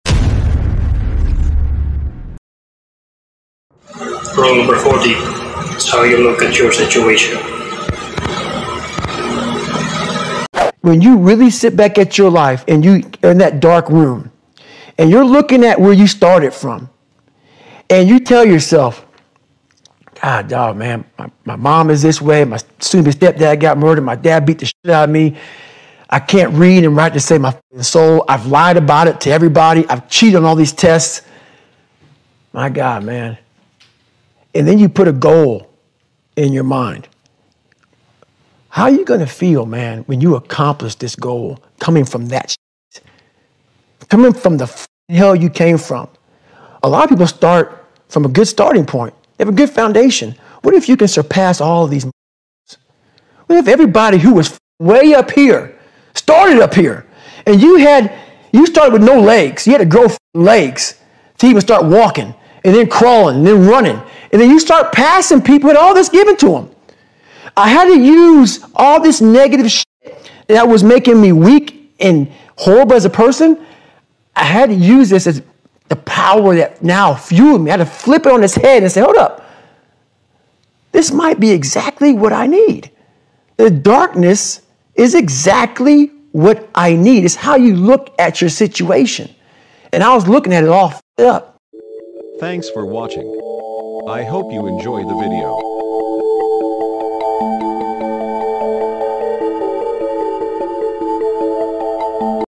#40 IT'S HOW YOU LOOK AT YOUR SITUATION | David Goggins | Driven
Put a goal in your mind, it doesn't matter if you have the tools you need or don't. And work on it and imagine how you gonna feel if you accomplish this goal coming from the shit you been. Use all the negative shit in your life in your life that's making you weak and horrible as a person, use that as a power that's going to fuel you up. It's how you look at your situation. Perception plays a big factor in life.